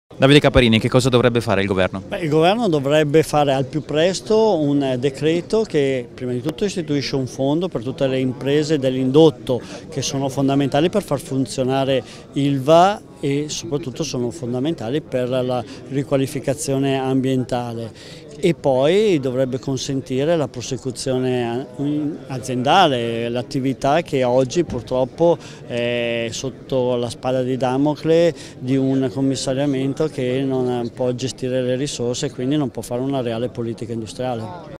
Ilva - Caparini: il governo tuteli le nostre aziende
ROMA, 13 FEB - “Le risorse necessarie per realizzare gli investimenti ambientali e produttivi necessari al rilancio di ILVA sono insufficienti. Infatti, per tornare a una situazione di pieno utilizzo della capacità produttiva serve oltre 1 miliardo di euro a cui vanno aggiunti 1,5 miliardi di euro necessari per la bonifica ambientale” Questo è quanto emerso in un incontro tra i rappresentanti di una decina di imprese dell’indotto ILVA di Brescia e Bergamo il deputato leghista Davide Caparini segretario di presidenza della Camera. Nel corso dell’incontro – spiega Caparini – “abbiamo analizzato le richieste di Confindustria che abbiamo trasformato in emendamenti dei senatori Consiglio, Stucchi e Arrigoni al decreto che sarà discusso dal Senato la prossima settimana – puntualizza il deputato camuno –.
Servono misure specifiche per tutelare le imprese dell’indotto
indispensabili per assicurare la continuità produttiva ed evitare
pregiudizi alla consistenza patrimoniale dell’ILVA. Il Decreto deve
quindi includere tra i crediti strategici scaduti che il commissario
straordinario può essere autorizzato a pagare anche quelli maturati
per la realizzazione di opere funzionali alla messa in sicurezza
ambientale. E’ inoltre fondamentale prevedere un canale di accesso privilegiato al fondo di garanzia per le PMI creditrici di ILVA, semplificando i requisiti di accesso al Fondo e innalzando le
percentuali di copertura e i massimali garantiti. I prossimi giorni –
conclude Caparini – saranno decisivi per il futuro di oltre 2mila tra
lavoratrici e lavoratori dell’indotto delle imprese bresciane e
bergamasche”.